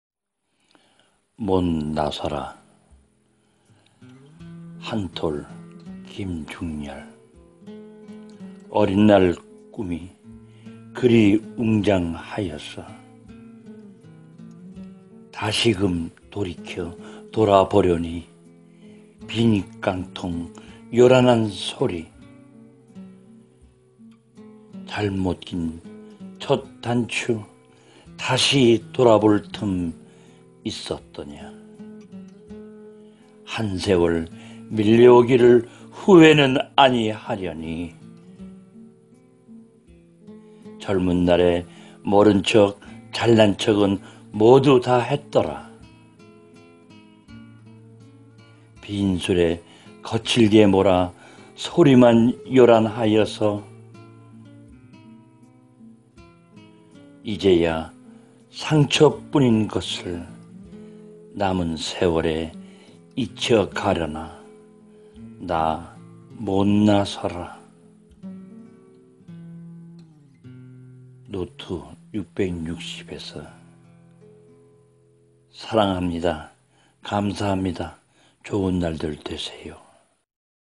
못 나서라 / 한톨 김중열 / 여울아라 15
《못 나서라》
               한톨 김중열

어린 날 꿈이
그리 웅장하여서
다시금 돌이켜 돌아보려니
빈 깡통 요란한 소리

잘못 낀 첫 단추
다시 돌아볼 틈 있었더냐
한 세월 밀려 오기를
후회는 아니하려니

젊은 날에 모른 척
잘난 척은 모두 다 했더라
빈 수레 거칠게 몰아
소리만 요란하여서

이제야
상처뿐인 것을
남은 세월에 잊혀가려나
나 못 나서라

[노트 660에서]